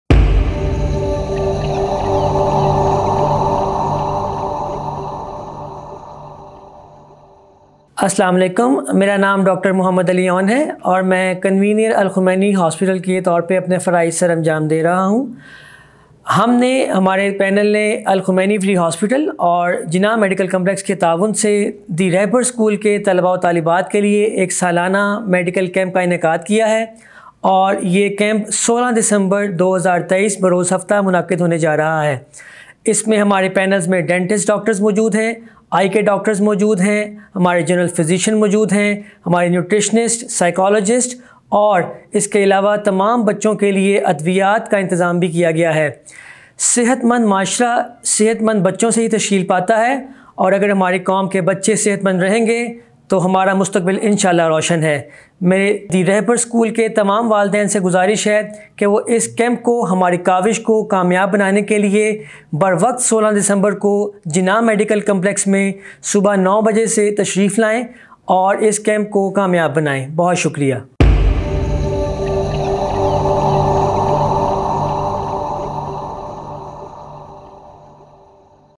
Dr. M, Ali Aoun Con. of Al Khumaini Hospital | Invitation For Free Medical Camp | At Jinnah Medical
Dr. Muhammad Ali Aoun | Convener of Al Khumaini Hospital | Giving
Invitation For The Free Medical Camp | At Jinnah Medical Complex

انجمنِ حسینیہ خواجگان نارووالی (رجسٹرڈ) لاہور
ٓAHKN